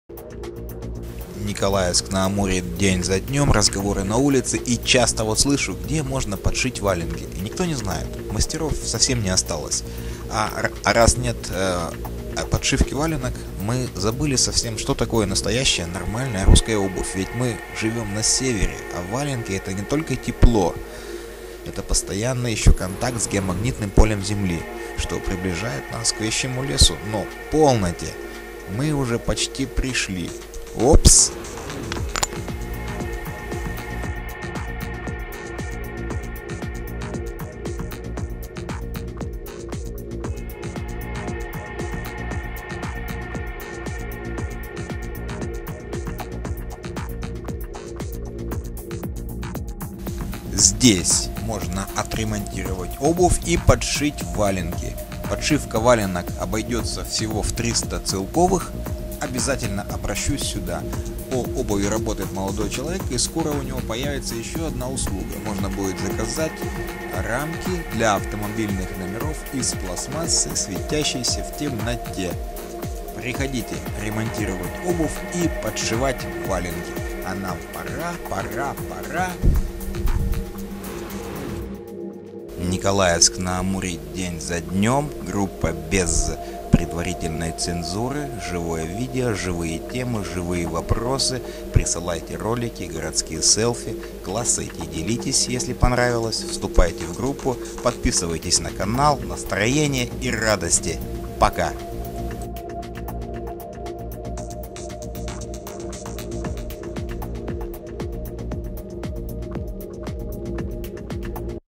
[Николаевск-на-Амуре ДЕНЬ ЗА ДНЁМ] [разГАворы на улице] #12 ЗДЕСЬ ПОДШИВАЮТ ВАЛЕНКИ!!!
где подшить валенки в Николаевске-на-Амуре ??